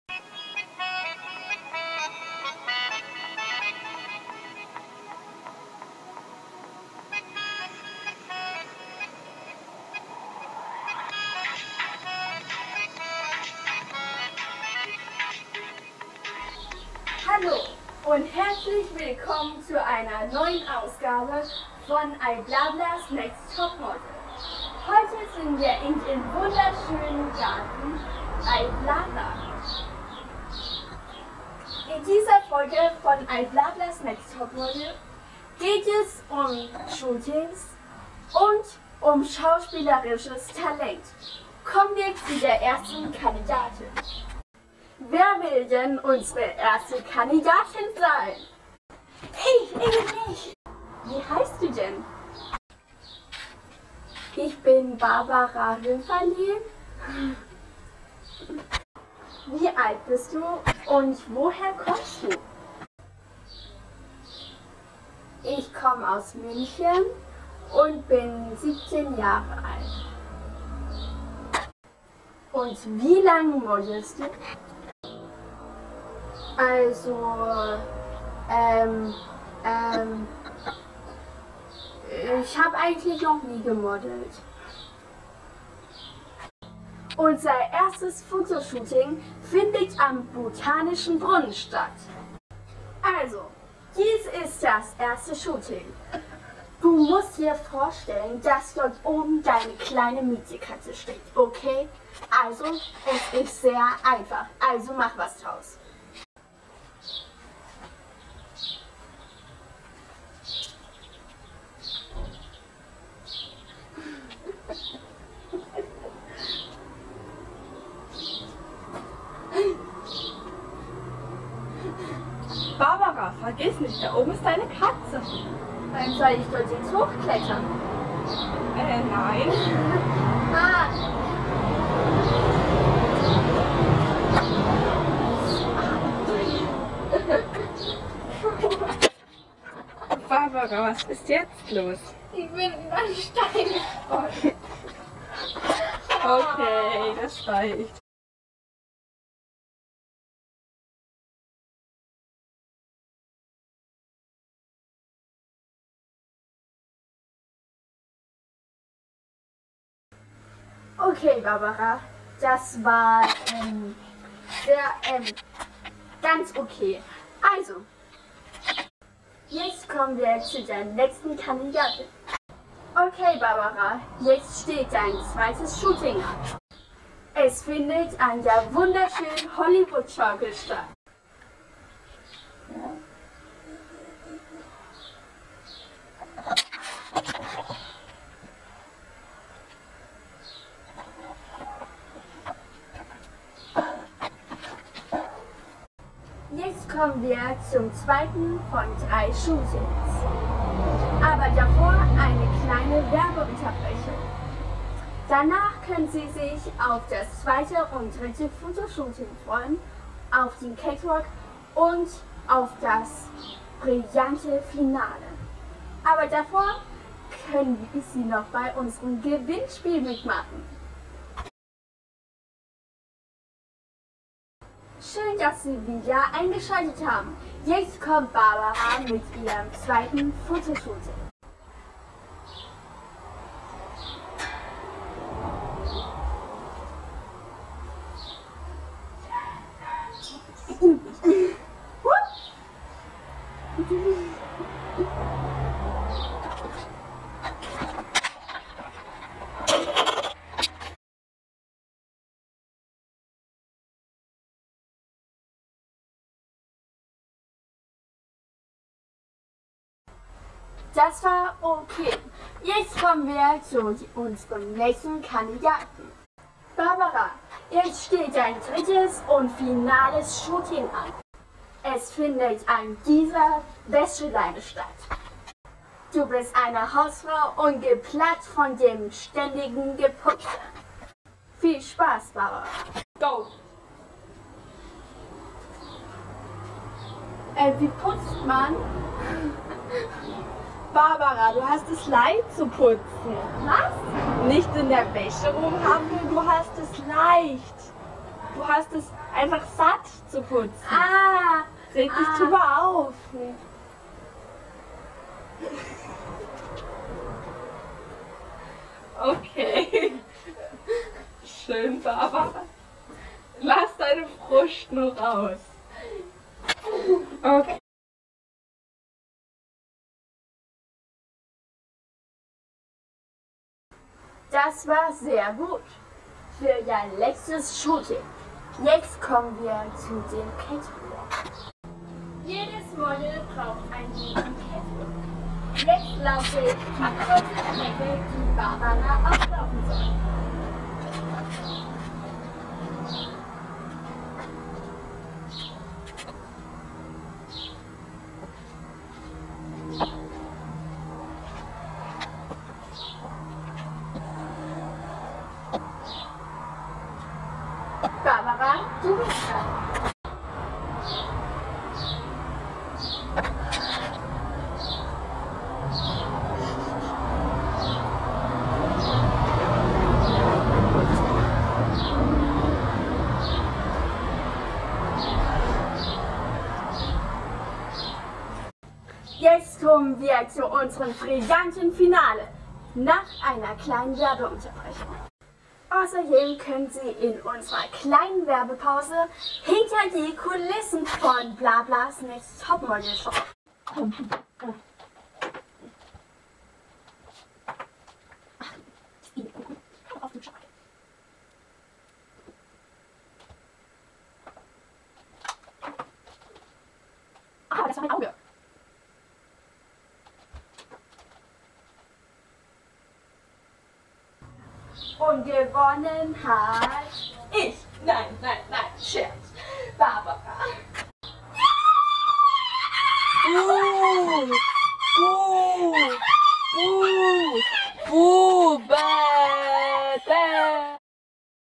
iBlaba´s next Topmodel 2050/Wie du als Model nicht posen solltest
Hi,
Barbara versucht als Topmodel berühmt zu werden :)